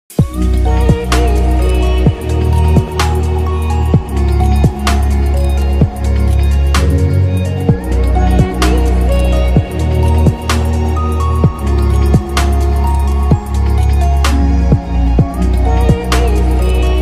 Ford Mustang Convertible Vibes with a Blonde Twist
lets save this world from suicide acts together with you ?
And ride with us as we hit the open road in a Ford Mustang Convertible, driven by a blonde beauty who embodies the spirit of freedom and adventure. 🚗💨 Join the journey as we explore scenic landscapes, feel the wind in our hair, and experience the undeniable allure of the iconic Mustang. Buckle up for a dose of excitement and style!